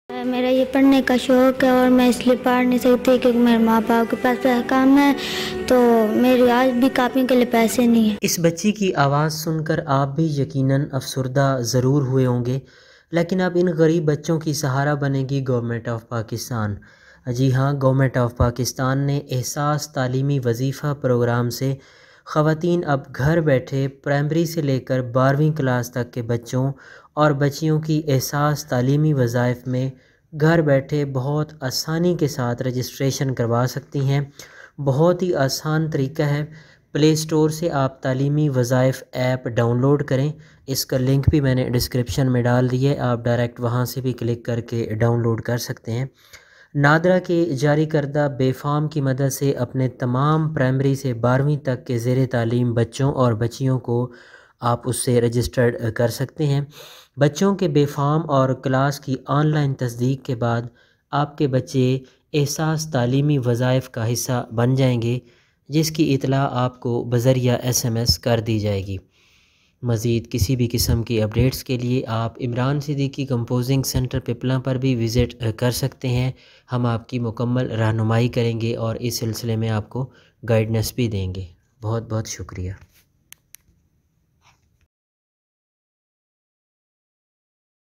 Ehsaas Taleemi Wazaif App launched by Govt of Pakistan, How to Use Ehsaas Taleemi Wazaif App
Govt of Pakistan has launched Ehsaas Taleemi Wazaif App for Pakistani nationals. Poverty Alleviation and Social Safety Division has launched this application. This application is available on the Google Play store for android phones.Primary school going boys get a quarterly stipend of Rs. 1,500 and girls Rs. 2,000; secondary school boys receive Rs. 2,500 and girls Rs. 3,000; and at higher secondary level, boys get Rs. 3,500 and girls Rs. 4,000. To-date, more than 6.7 million children have been enrolled in this program.
Government Launch New App For Ehsaas Taleemi Wazaif Registration | Ehsaas Taleemi Wazaif App
The Ehsaas Taleemi Wazaif program has been set up for cross country execution after difficult work and various contributions throughout the most recent 3 years. At last, the program expects to help the schooling of 10 million youngsters across all Tehsils of the country. Kids matured 4 to a long time from all Ehsaas qualified families can enlist for the school payments of Ehsaas. This program incorporates essential, auxiliary and higher optional school payments for youngsters to work on their admittance to instruction. In accordance with the Ehsaas’ weighted for young ladies allowances strategy for all restrictive money move programs, young ladies get a higher payment across all age gatherings.
Imran Siddiqui Piplan